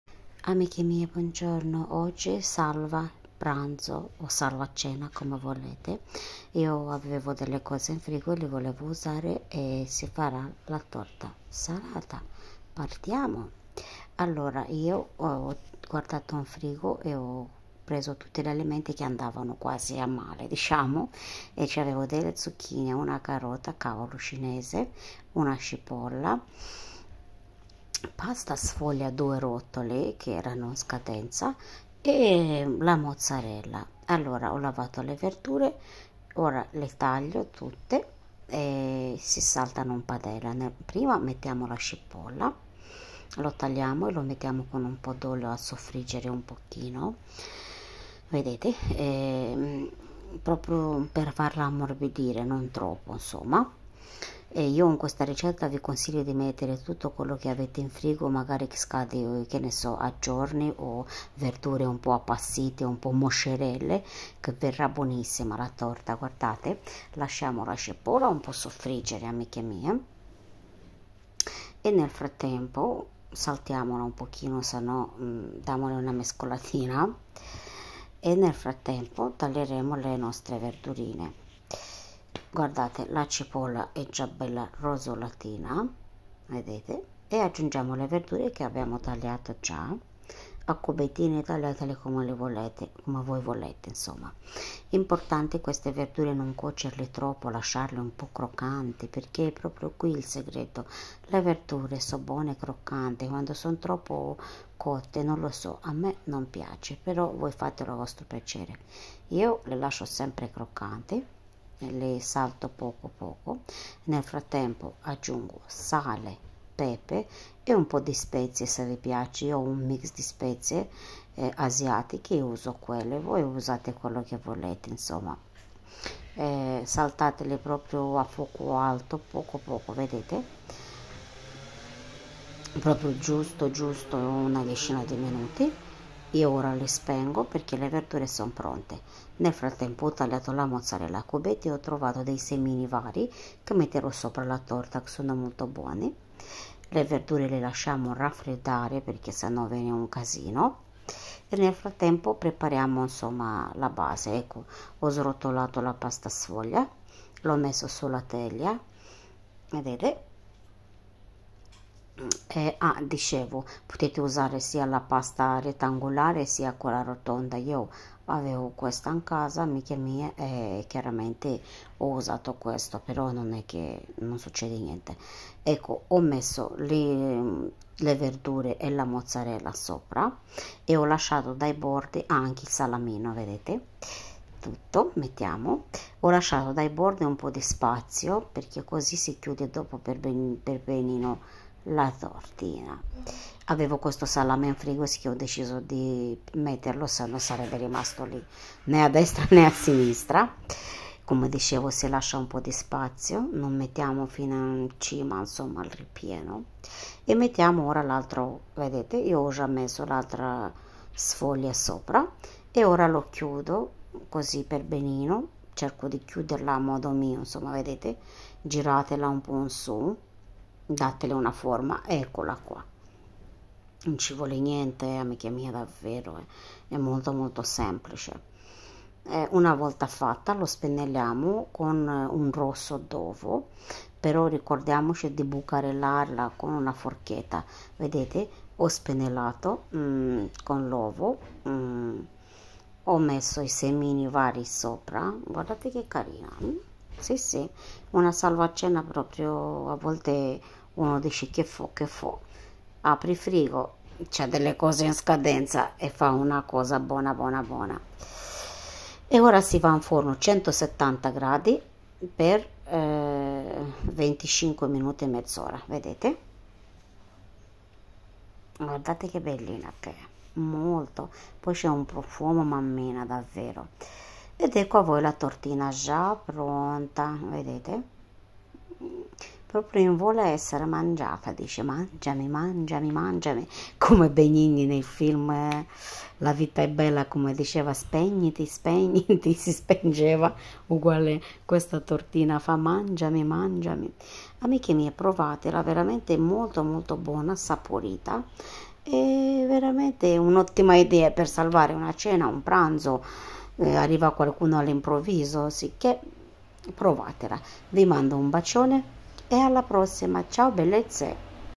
Torta salata. (Questa ricetta si chiama salva pranzo/cena. Con tutto quello che troviamo nel frigo)
Amiche mie non scrivo l'ingredienti perche e una  ricetta svolta frigo, sicché fatela con quello che trovate e a vostro piacere.😋😋😋